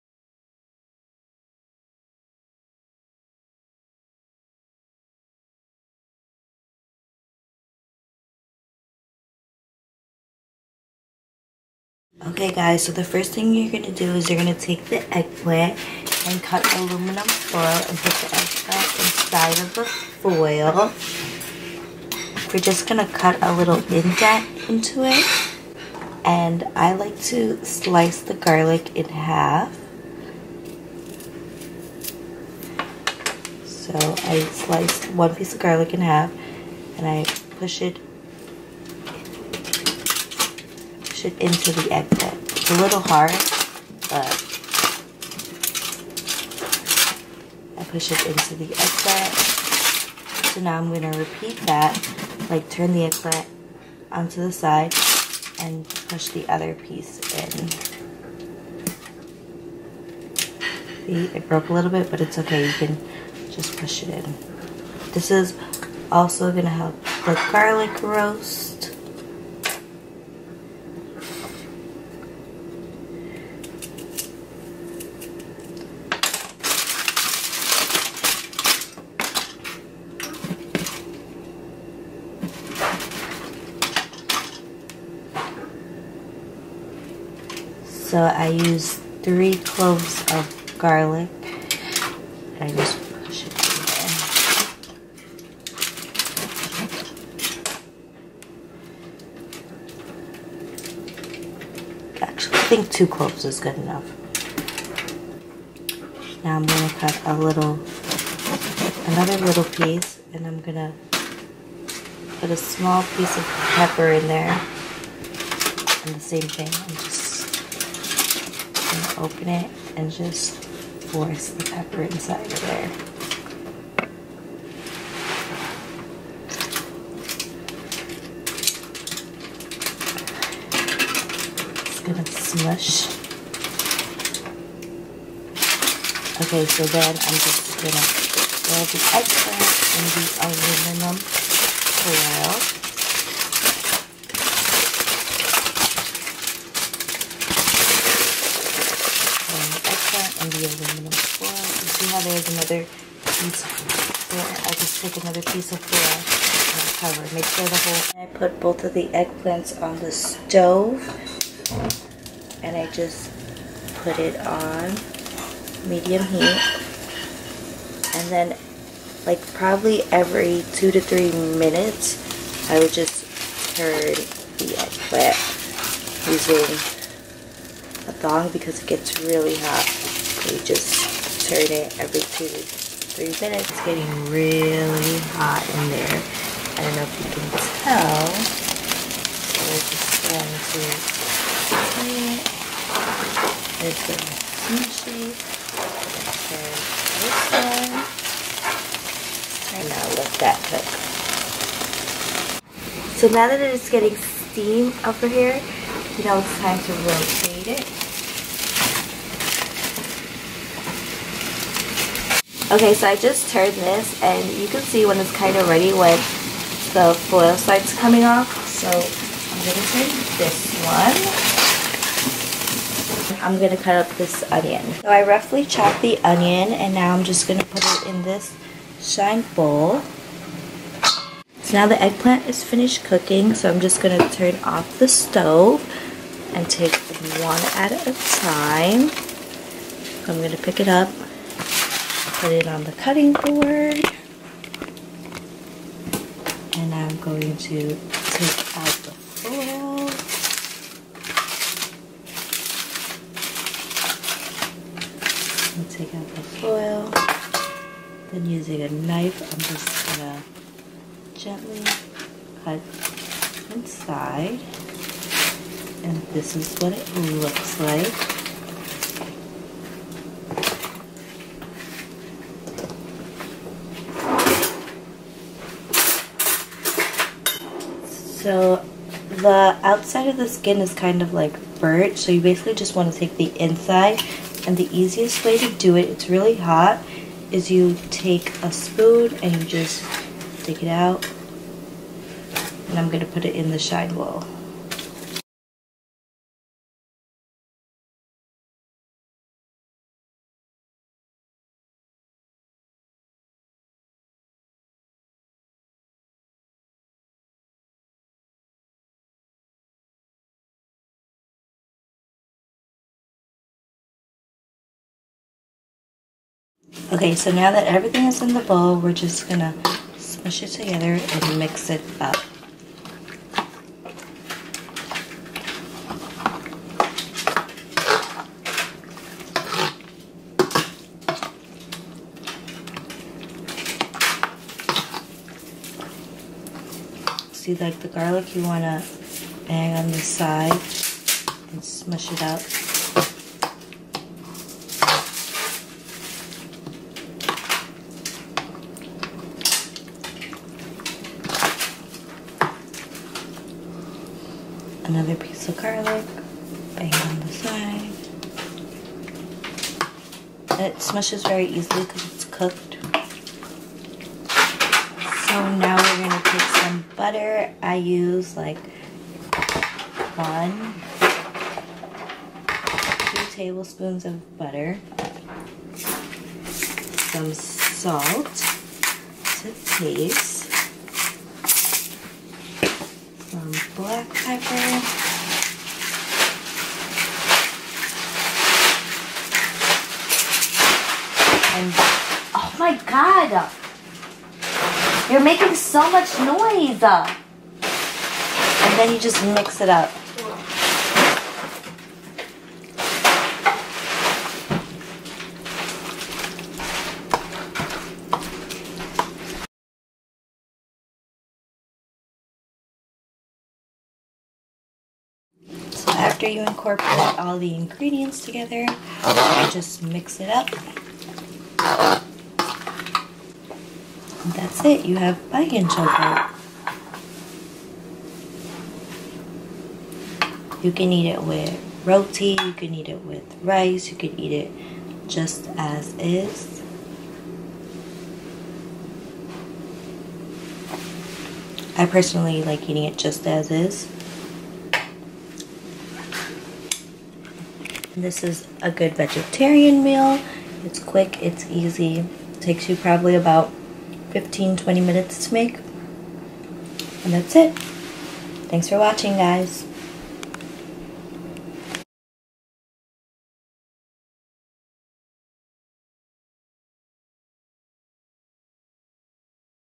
Cooking: Bigan/ Eggplant Choka
This video is about Cooking: Bigan Choka

Ingredients:
Eggplant
Garlic
Onion
Pepper
Butter
Salt
Pepper

Love Always
Jaya Ramkumar